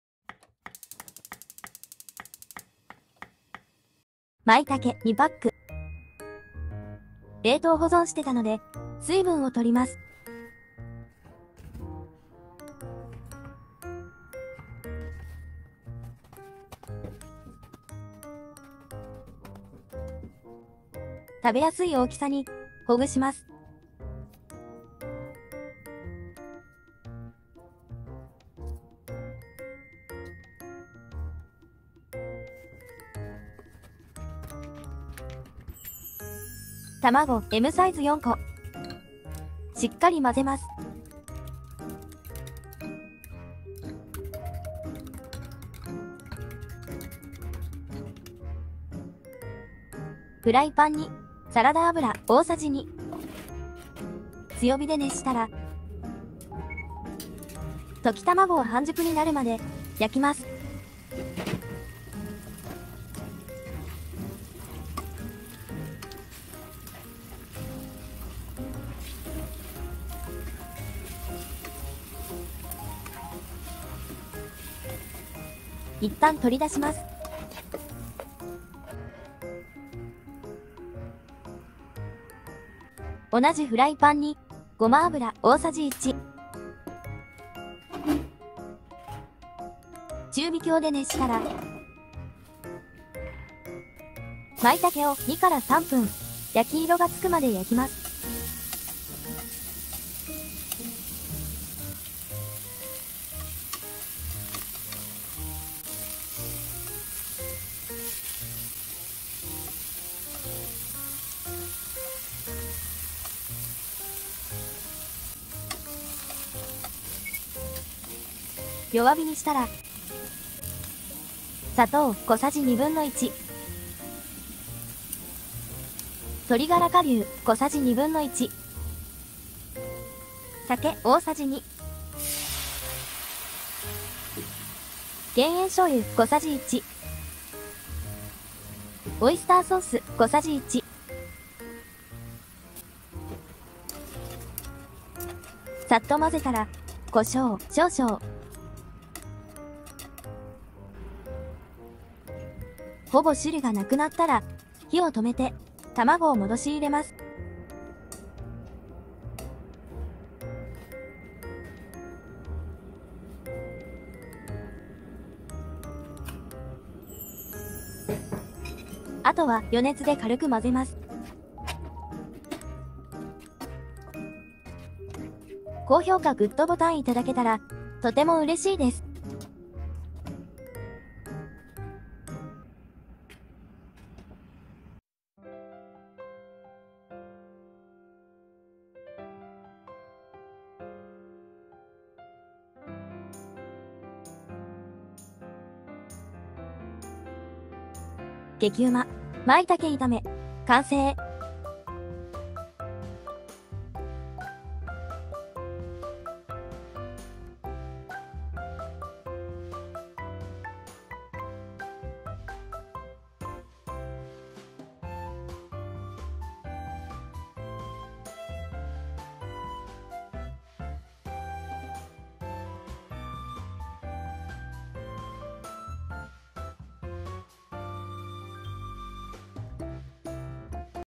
【舞茸レシピ】とろ～り卵と絡み合ったら最高に美味しい炒めおかず。
【舞茸レシピ】舞茸を焼き色がつくまで焼いて半熟の卵と混ぜ合わせるだけの簡単おかず

舞茸【2パック】
卵【Mサイズ4個】

〈調味料〉
砂糖【小さじ1/2】
鶏ガラ顆粒【小さじ1/2】
酒【大さじ2】
減塩醤油【小さじ1】
オイスターソース【小さじ1】
コショウ【小さじ1】

毎週水曜日·土曜日
午後4時に最新動画配信しています🙋

よろしければ高評価👍グッドボタン頂けたらとても嬉しいです🙇

◆Amazonリンク◆

『おすすめ🍴レシピ』